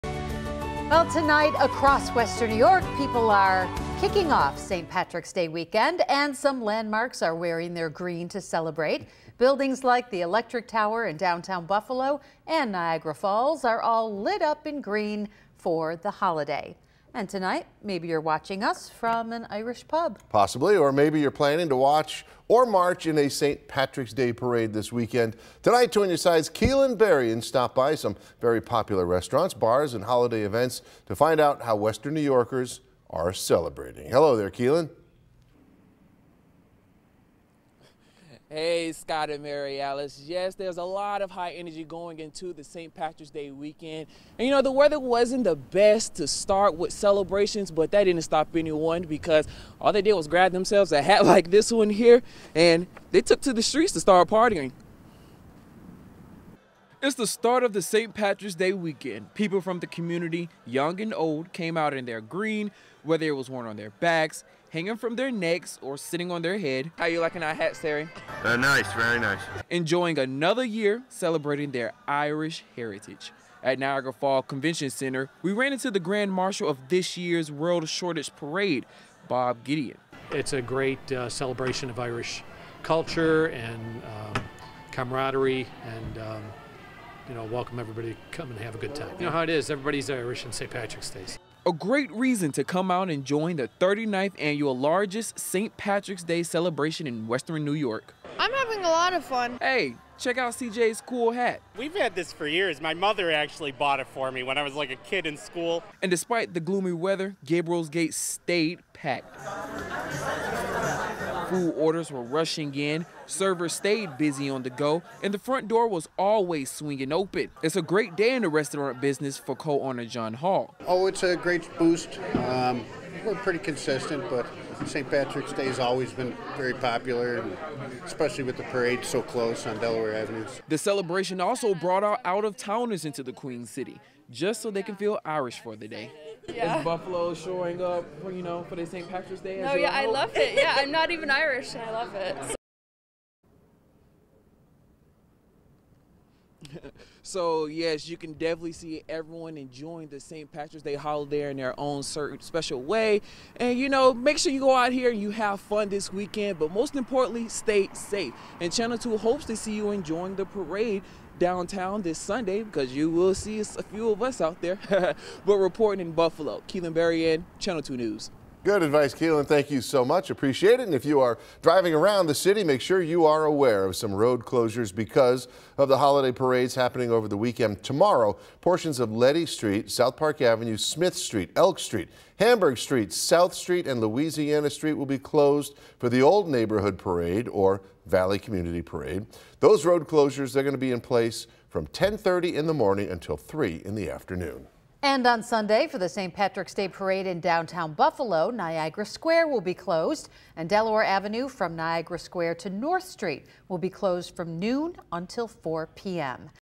St. Patrick's Day weekend brings many Western New Yorkers out to party
It's the start of St. Patrick's Day weekend. People from the community, young and old, came out in their green, whether it was worn on their backs, hanging from their necks, or sitting on their heads, enjoying another year celebrating their Irish heritage on St. Patrick's Day.